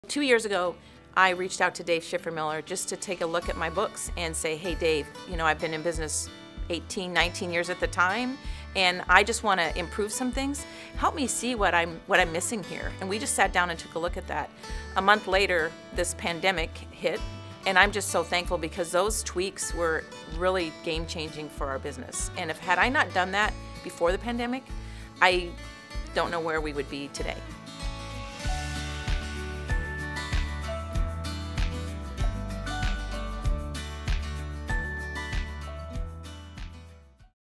Cornhusker Bank and Sherry Tafoya, Custom Blinds & Design - Partnership and Pandemic Response
Sherry Tafoya, Founder of Custom Blinds and Design, shares how the Cornhusker Bank team helped her enhance her business model which helped her prepare for the effects of the Pandemic. And why she puts her trust in the team at Cornhusker Bank!